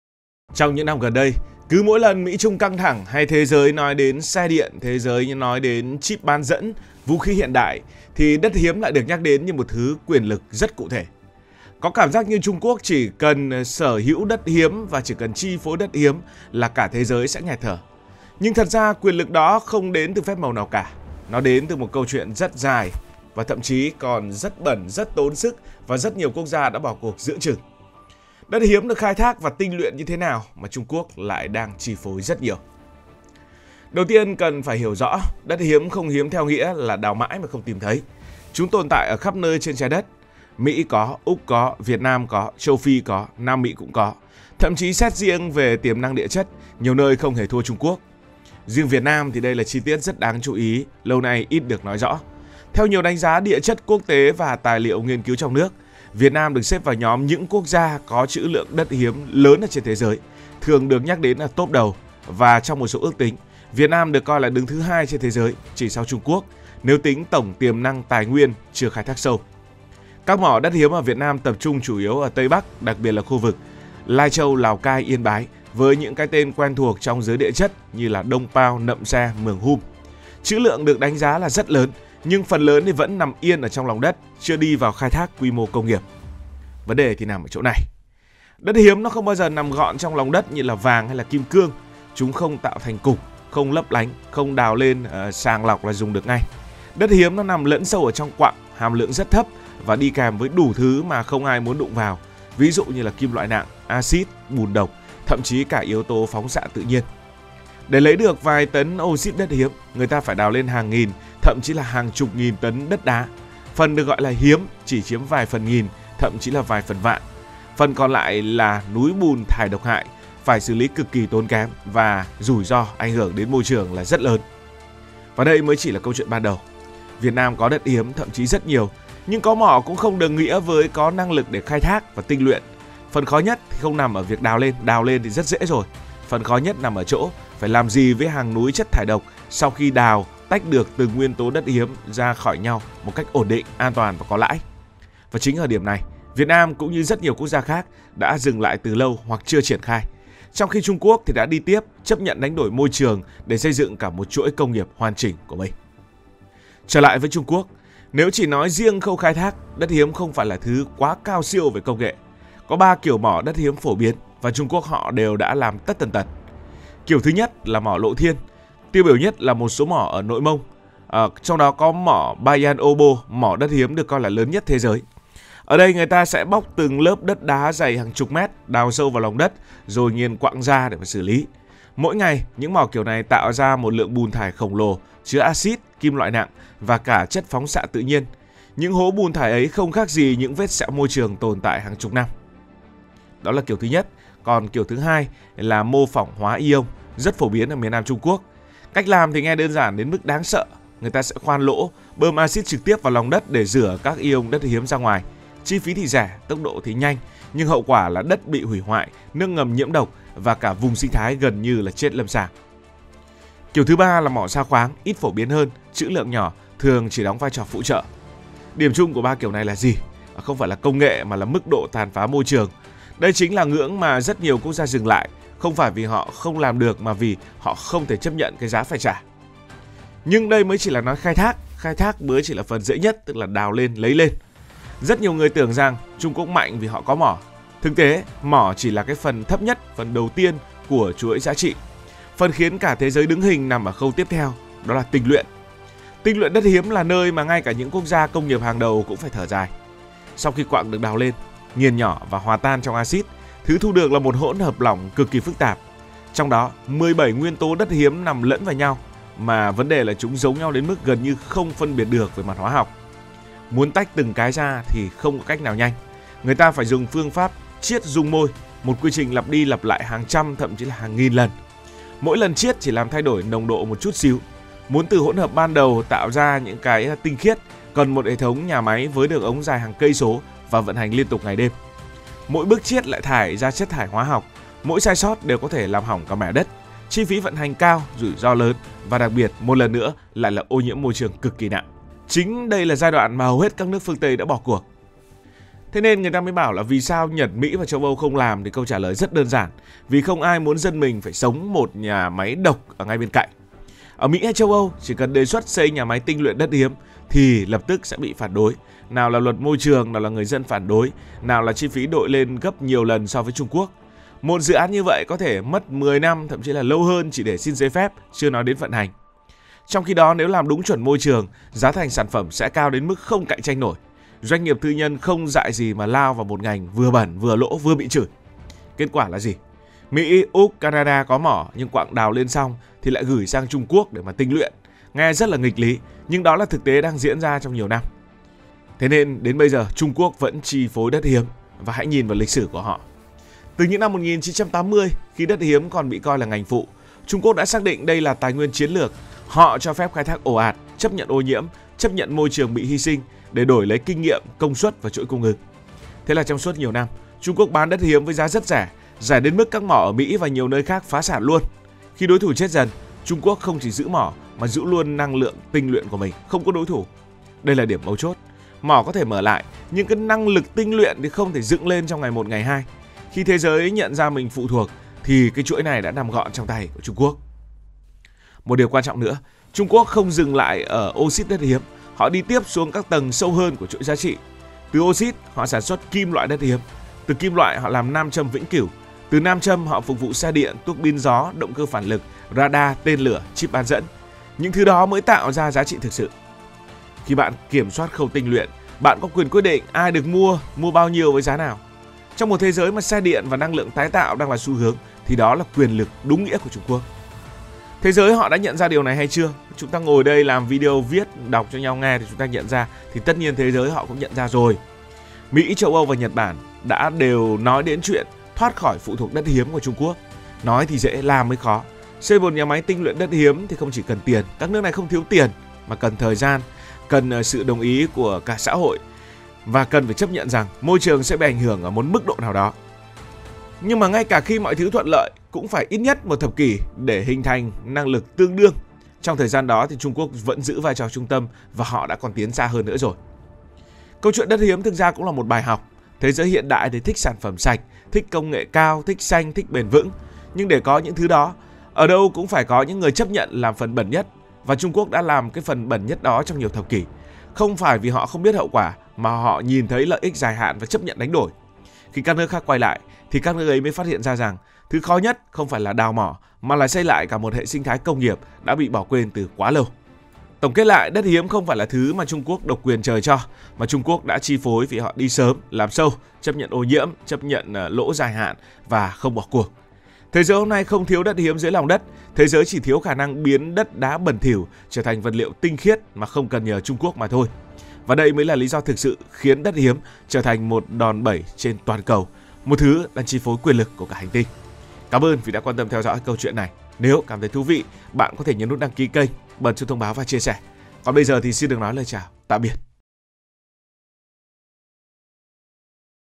ĐẤT HIẾM KHÓ KHAI THÁC RA SAO? VÌ SAO TRUNG QUỐC ĐỘC TÔN?
Đất hiếm không hiếm về trữ lượng, nhưng cực kỳ khó khai thác và tinh luyện. Trung Quốc chi phối không phải vì có nhiều mỏ nhất, mà vì họ đi trọn con đường từ khai thác bẩn, tinh luyện phức tạp đến sản phẩm công nghiệp cuối cùng. Câu chuyện đất hiếm là bài học về chuỗi giá trị, sự đánh đổi môi trường và quyền lực công nghiệp trong thế kỷ 21.